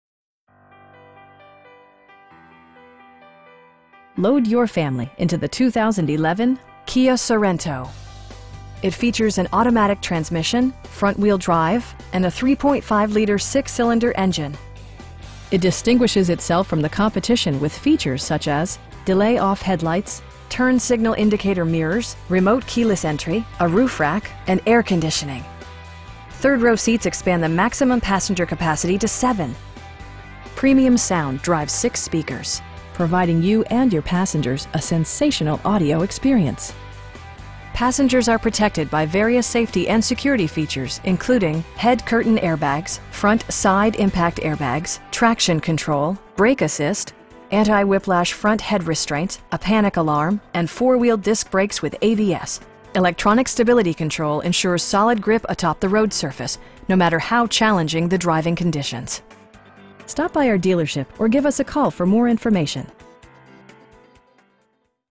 2011 Kia Sorento SUV in Oklahoma City, OK 73114
Bob Moore Kia
13020 N. Broadway Extension

Load your family into the 2011 Kia Sorento. It features an automatic transmission, front-wheel drive, and a 3.5 liter 6 cylinder engine. It distinguishes itself from the competition with features such as: delay-off headlights, front bucket seats, turn signal indicator mirrors, remote keyless entry, an overhead console, a roof rack, and air conditioning. Third row seats expand the maximum passenger capacity to 7. Premium sound drives 6 speakers, providing you and your passengers a sensational audio experience. Passengers are protected by various safety and security features, including: head curtain airbags, front SIDE impact airbags, traction control, brake assist, Anti-whiplash front head restraints, a panic alarm, and 4 wheel disc brakes with ABS. Electronic stability control ensures solid grip atop the road surface, no matter how challenging the driving conditions. We have the vehicle you've been searching for at a price you can afford. Stop by our dealership or give us a call for more information.